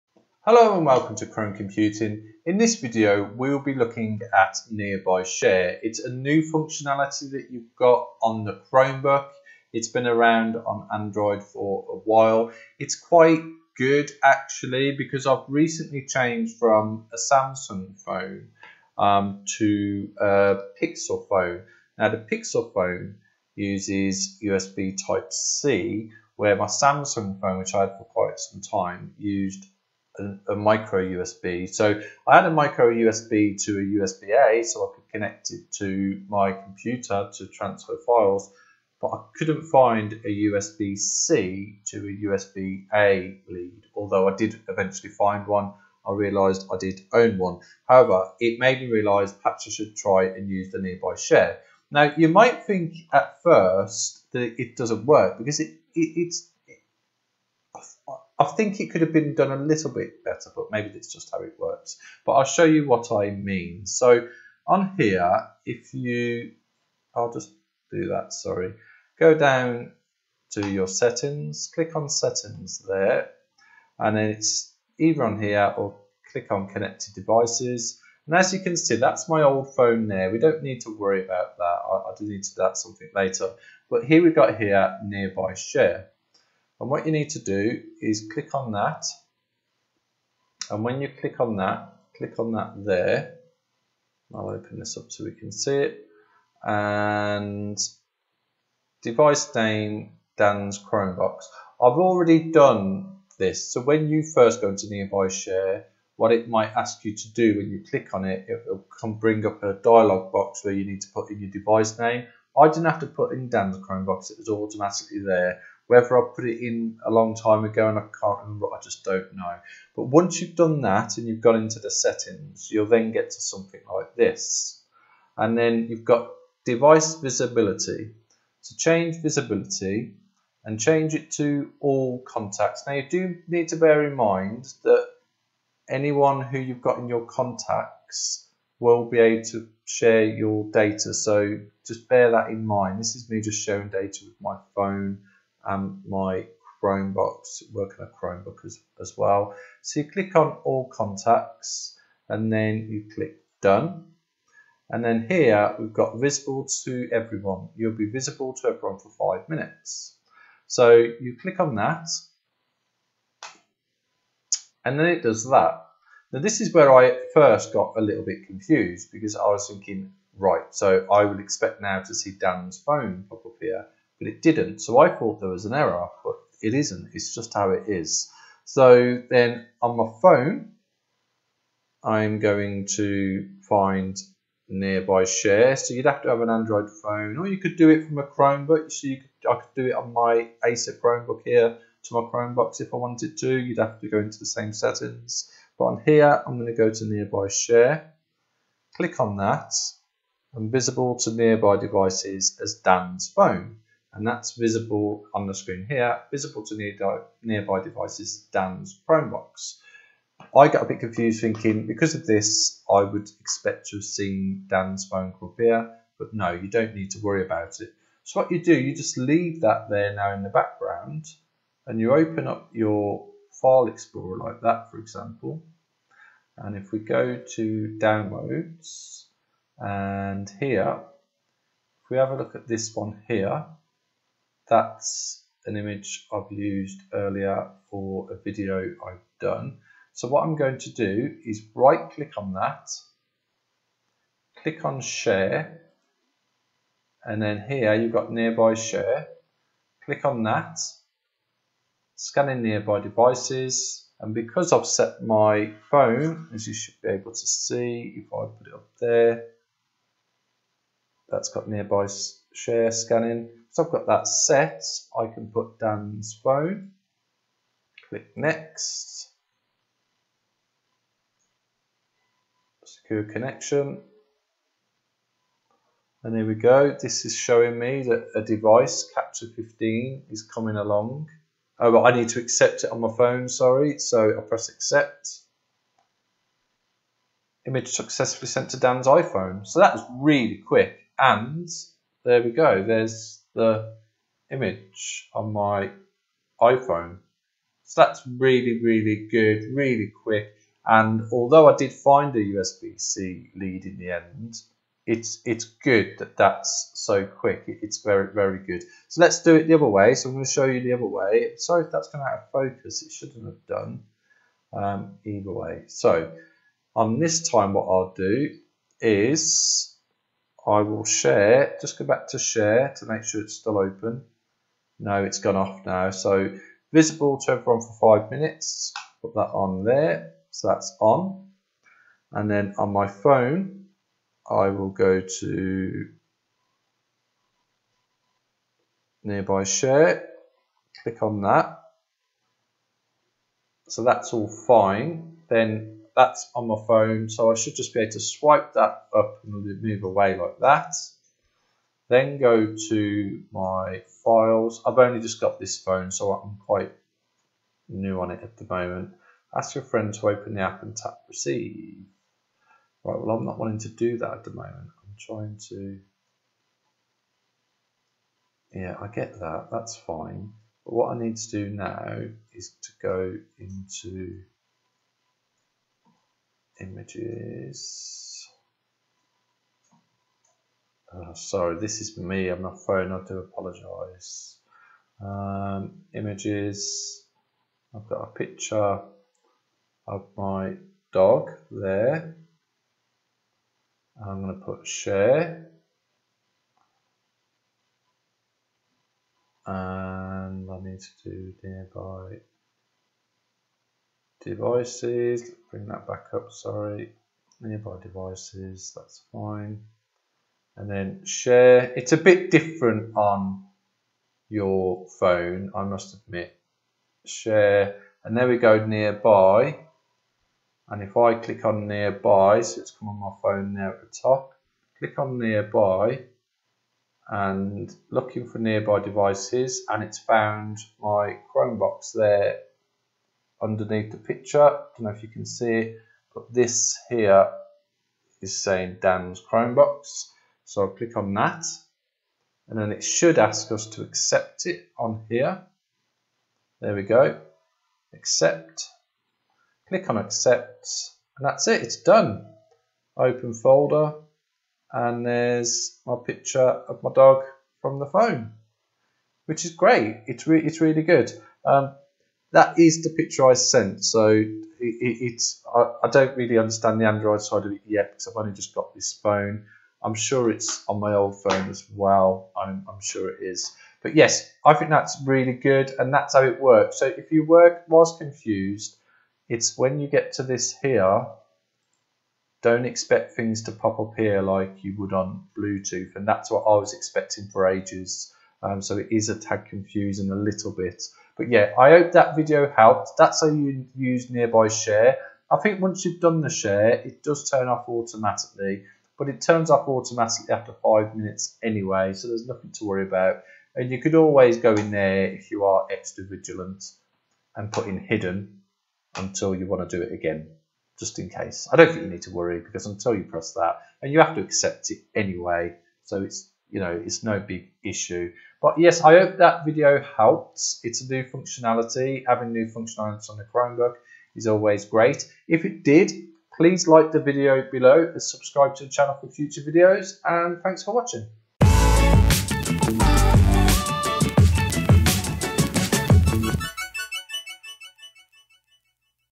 What is Nearby Share and how to use it on your Chromebook
There is a new functionality available on the Chromebook called Nearby Share. If you're unsure what it is or want to know how it works then this video is for you.

In this video I'll show you how to set up Nearby Share on your Chromebook and how to share a file from your Chromebook to your Android Phone. I'll also show you how to share a file from your Android phone to your Chromebook.